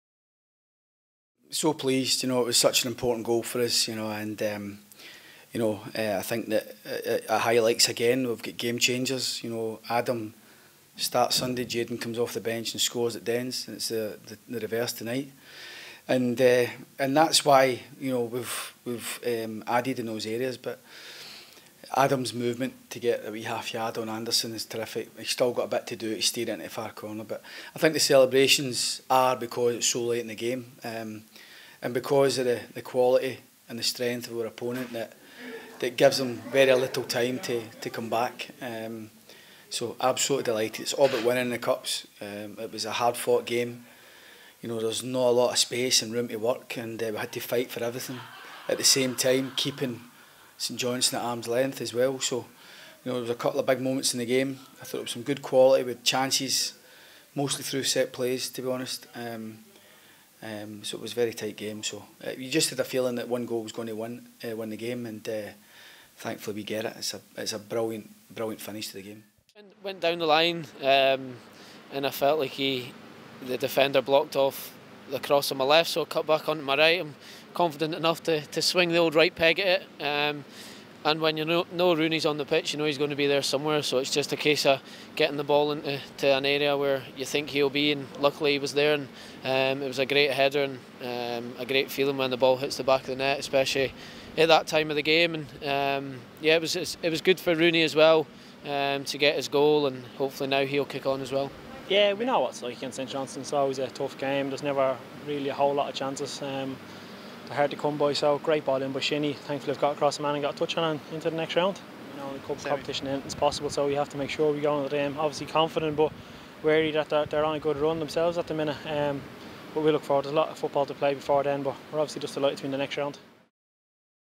Betfred Cup QF | Reaction Round Up
Derek McInnes, Graeme Shinnie and Adam Rooney give their reaction to The Dons 1-0 victory over St Johnstone in the Betfred Cup Quarter Final